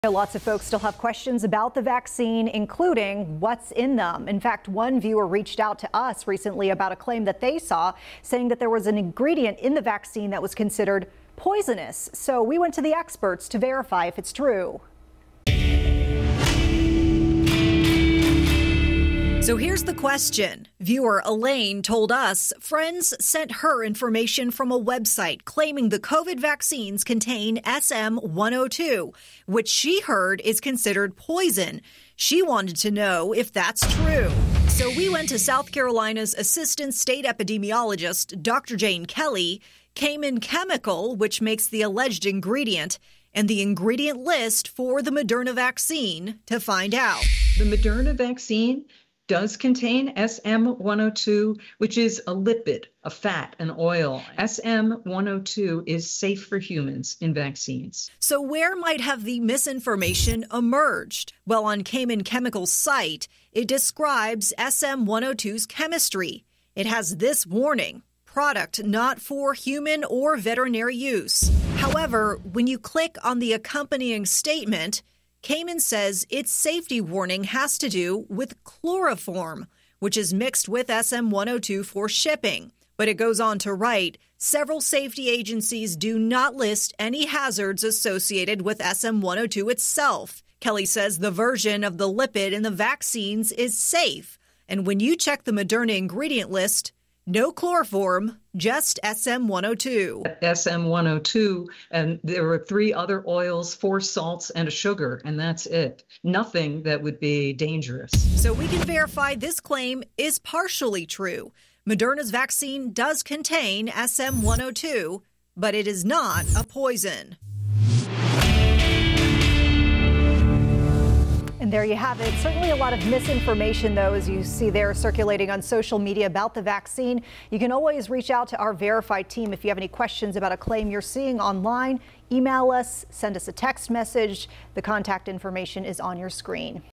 VERIFY: Does the COVID-19 vaccine have SM-102? Yes, and it's safe
Some people still have questions about the COVID-19 vaccines, including questions about what is in them. An online claim states that the COVID-19 vaccines contain a substance not suitable for humans, but experts say that's not true.

The Question
Do any of the COVID-19 vaccines contain SM-102? Is SM-102 a poison?

The Answer
Yes, Moderna's vaccine contains SM-102, but SM-102 is not considered dangerous.